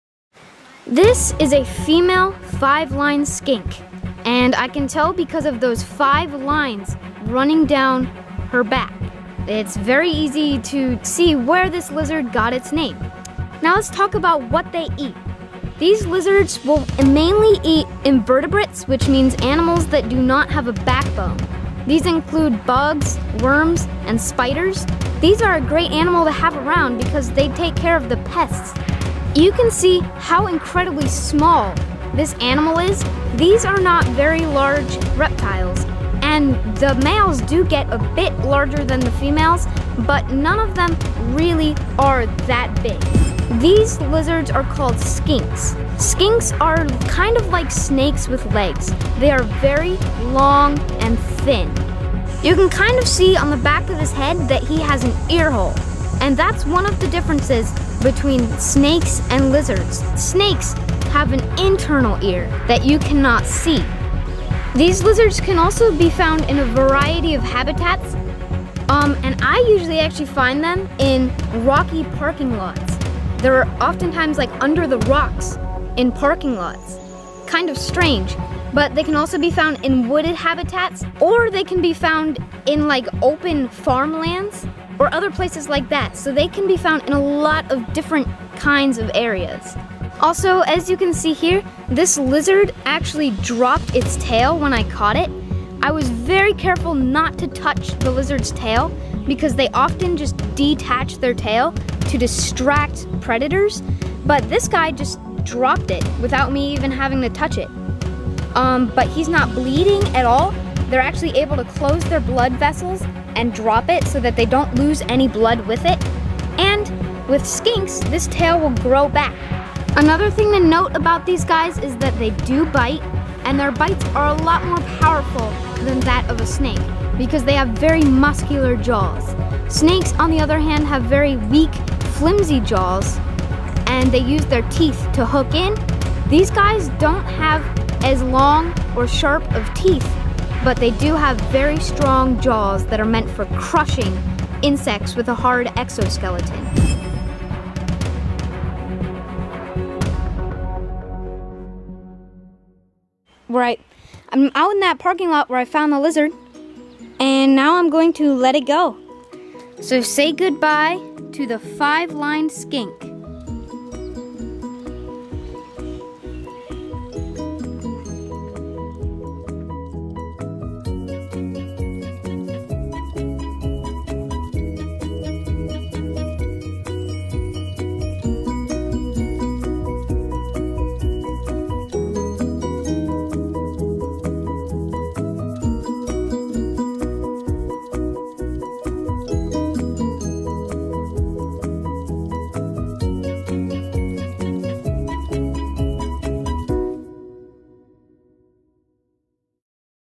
Catching a five-lined skink!
All of the basic information you need to know about this adorable little lizard!!!